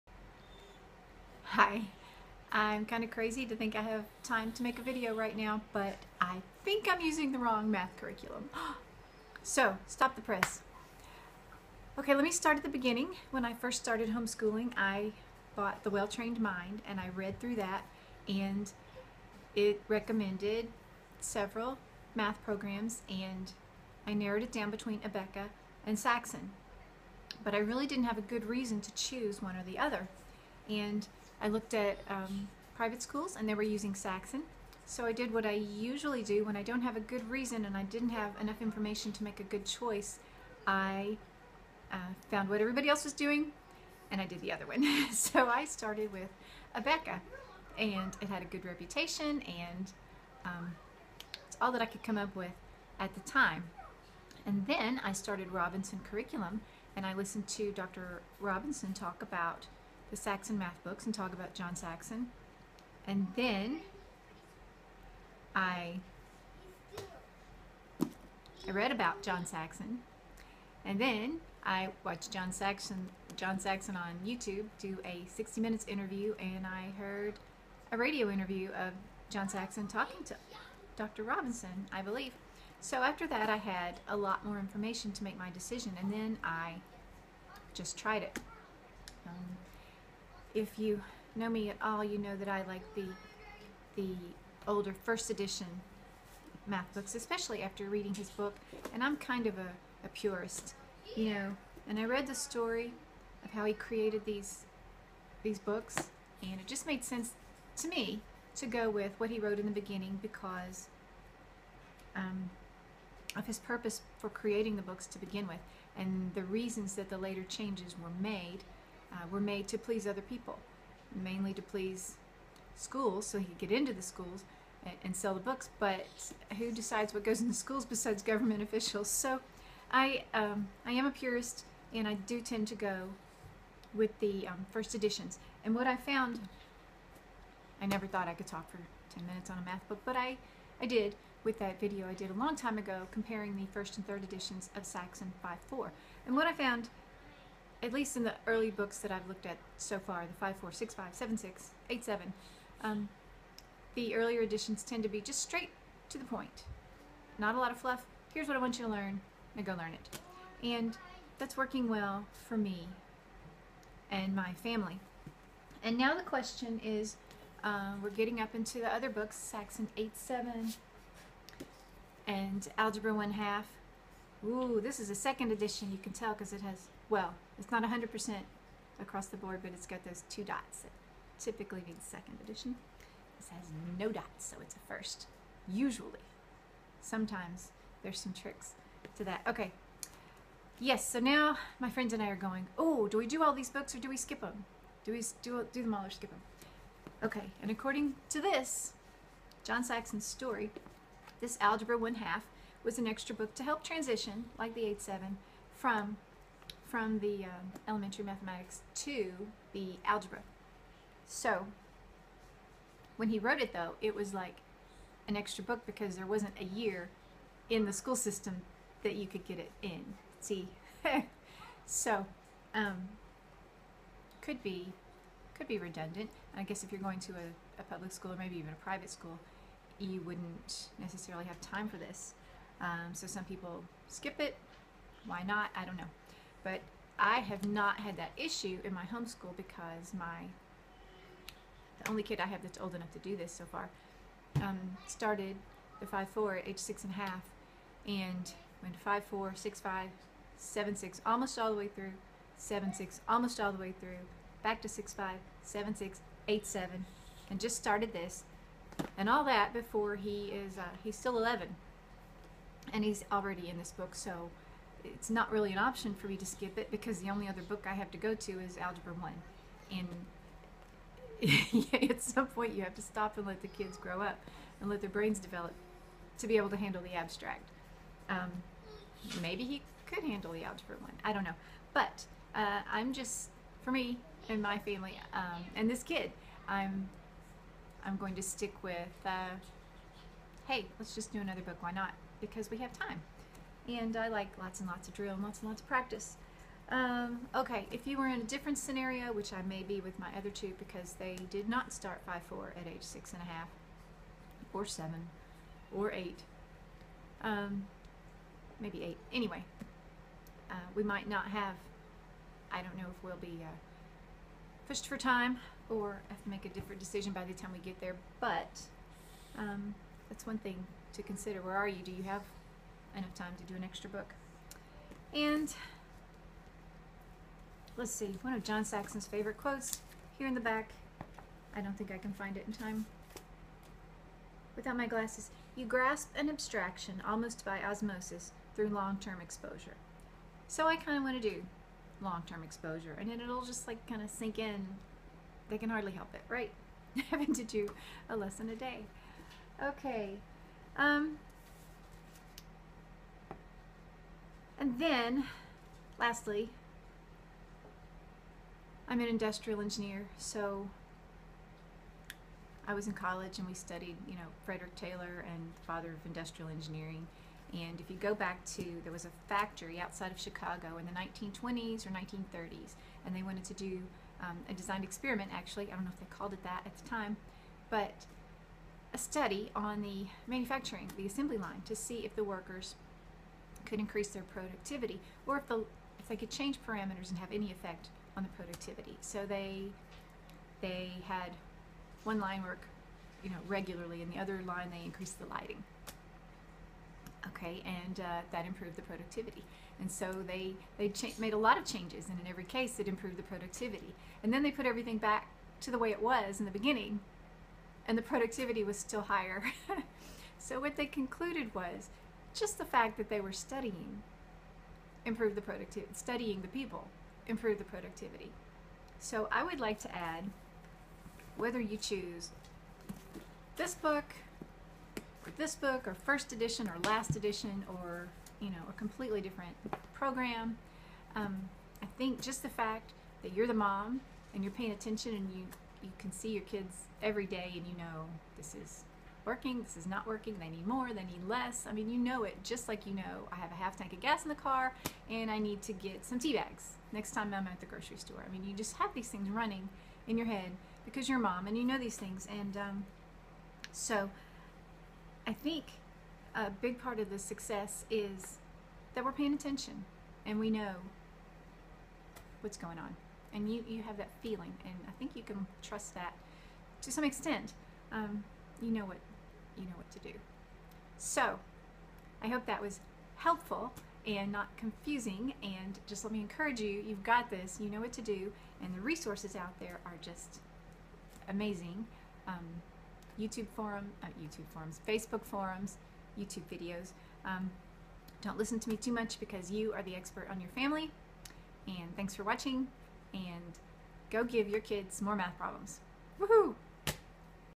Homeschool Math: The Absolute Best Curriculum :)
Just doing what homeschool moms do -- over-analyzing every decision. Here is some perspective:)